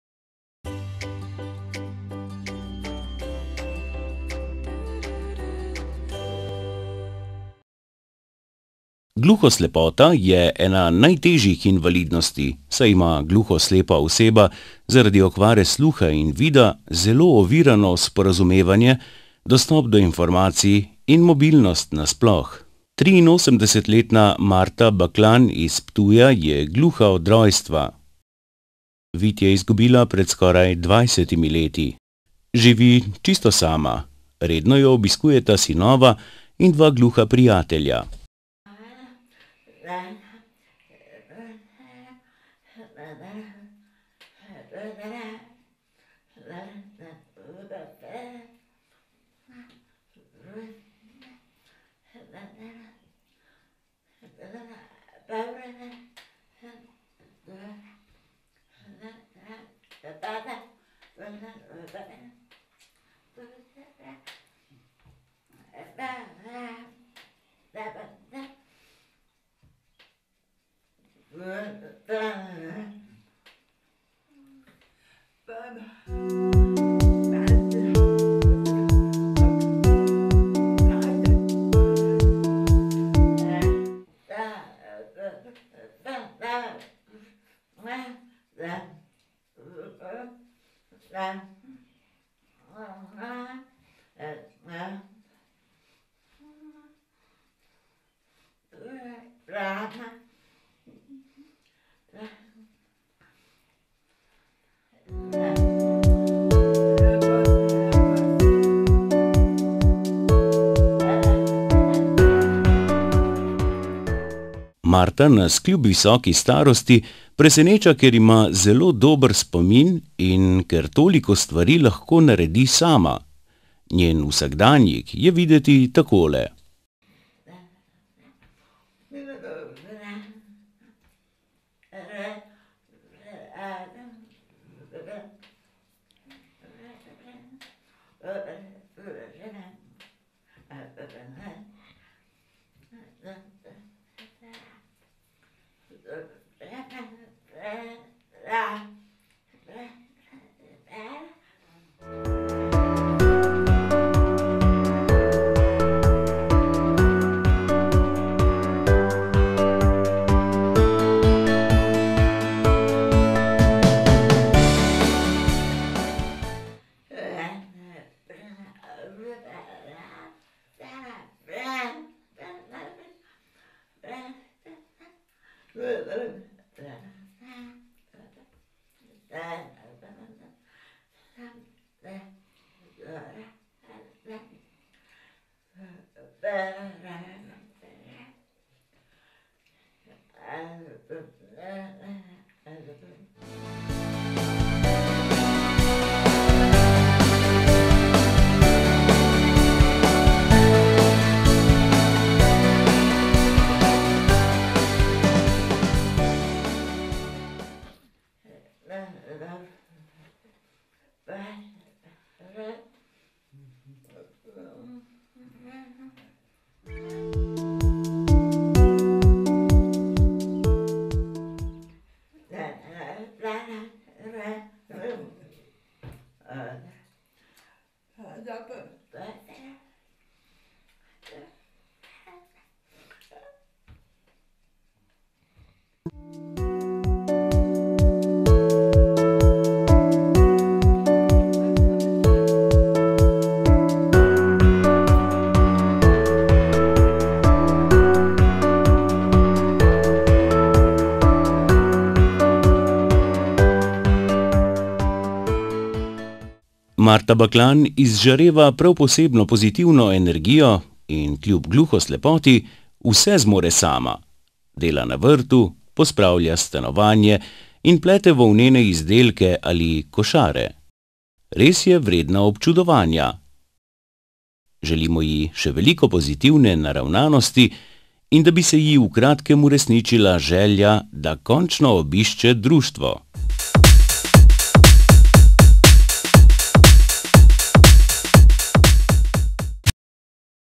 Marta kljub gluhoslepoti živi povsem običajno življenje
V Evropski uniji živi okoli 150.000 gluhoslepih državljanov, ki so ena najbolj izključenih manjšin. Pogovarjali smo se z gluhoslepo Marto Baklan iz Ptuja, ki kljub izgubi sluha in vida ni obupala. Živi povsem običajno življenje in vse dela sama brez pomoči drugih.